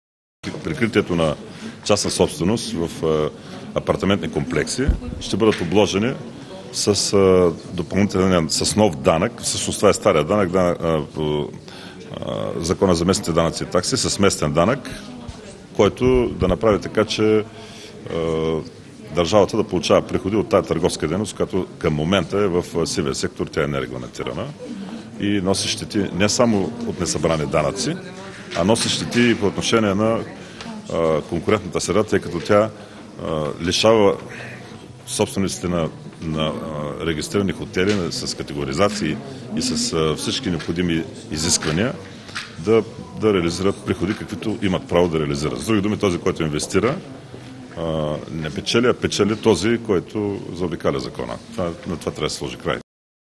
Вицепремиерът Симеонов ще спре сивата икономика при апарт-хотелите
Вицепремиерът Валери Симеонов ще въведе регулация чрез законодателни промени за плащането на такси от така наречените апарт-хотели, които досега бяха в сивата икономика. Ето и подробностите за предвидения от НФСБ законопроект